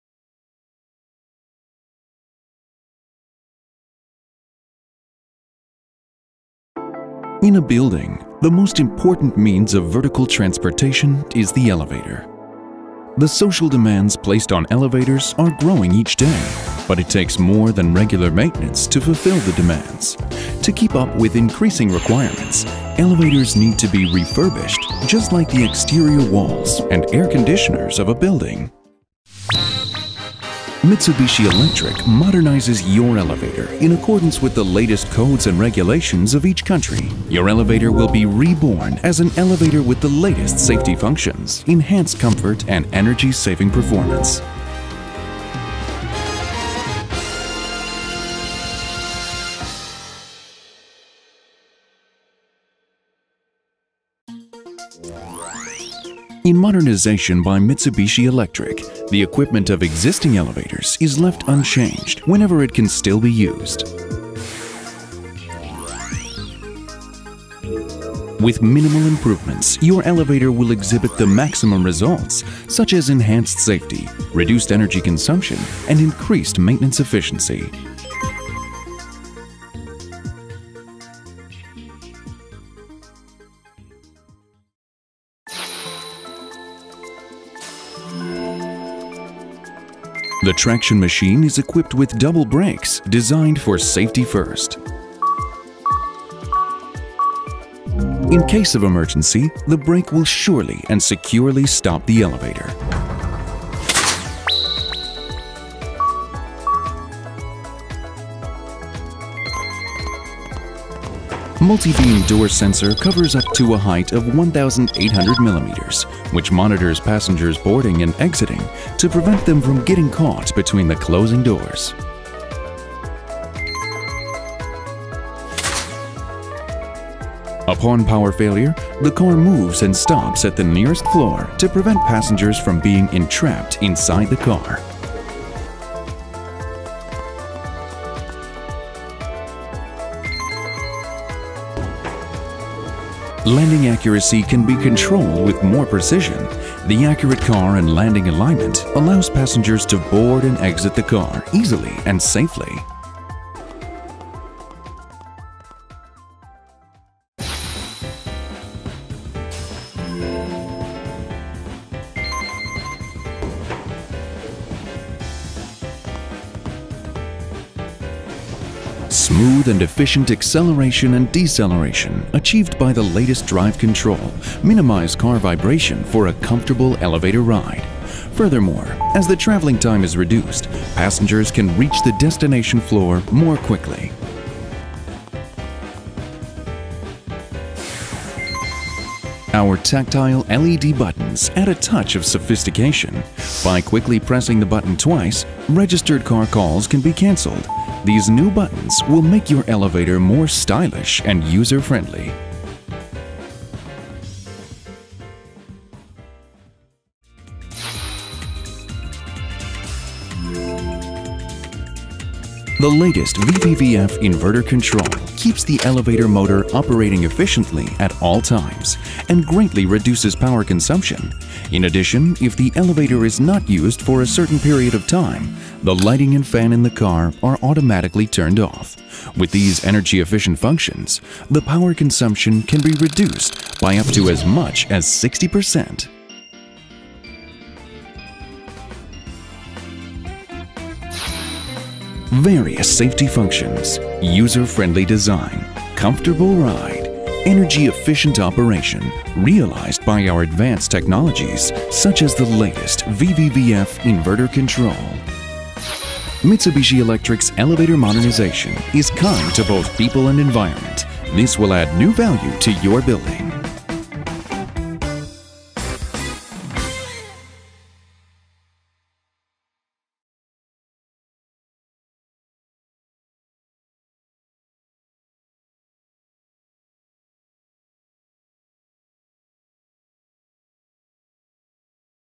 [English] Elevator Modernization [Mitsubishi Official Video]
Even with proper maintenance and normal operation, elevator components progressively deteriorate over a long period of use. To ensure passenger comfort and overall safety, elevator system modernization is required.

Specifications subject to change without notice.